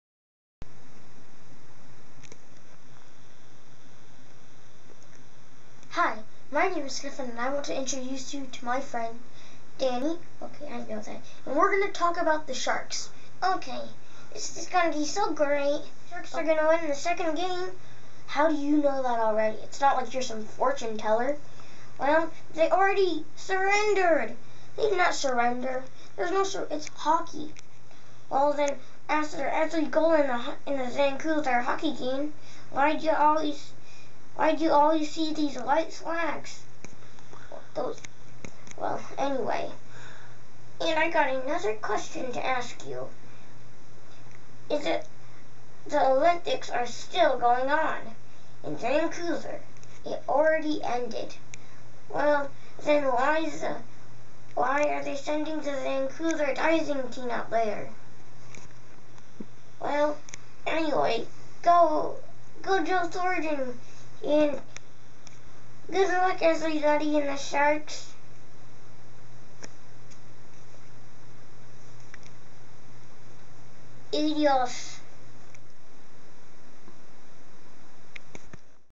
Sharks v Vancouver: Game 2 Stefan ventriloquist preview
Stefan and his dummy, Danny, do a preview of Game 2 of the Sharks v Vancouver 2011 playoffs.  Stefan the ventriloquist has a little fun with the Canucks.  Go Thornton and the Sharks.